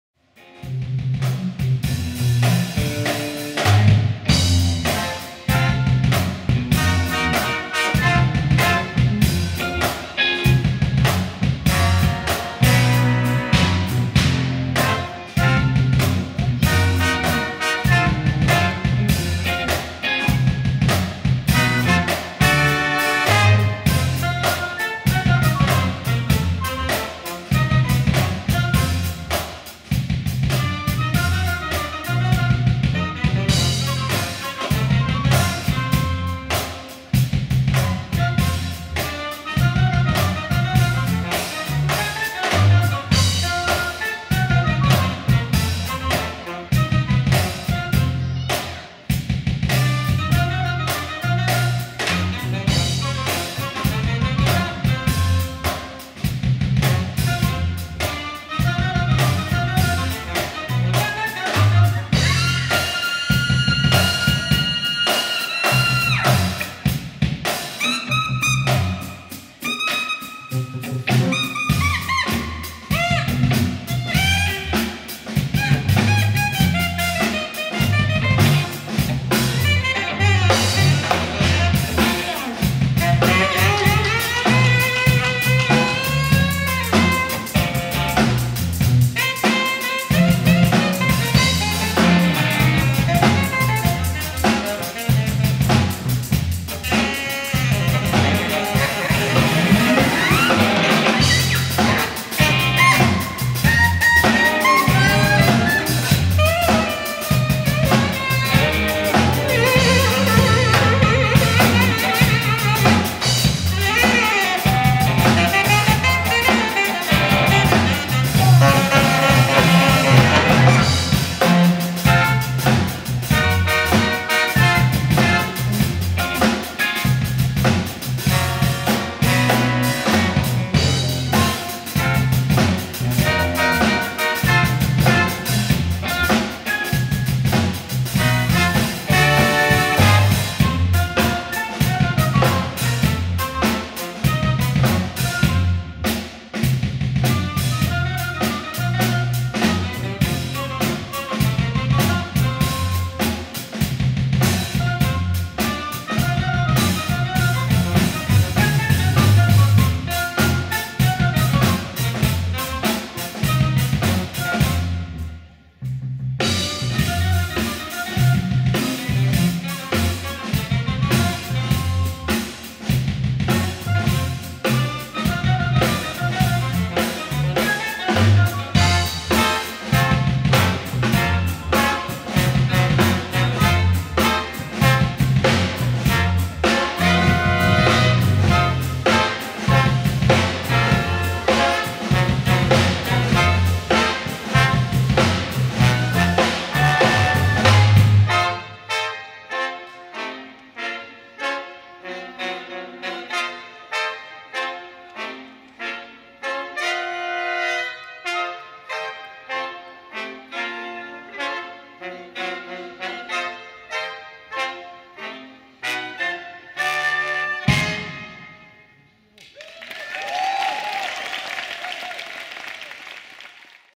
Vincent Pongracz Synesthetic Octet / 20 Jahre Porgy&Bess - Street Jazz Festival 'Mixed Pickles'
Vincent Pongracz: clarinet
Doris Nicoletti: flute, piccolo flute
Clemens Salesny: reeds
Richard Köster: trumpet
Alois Eberl: trombone
Peter Rom: guitar
Manuel Mayr: bass
Lukas König: drums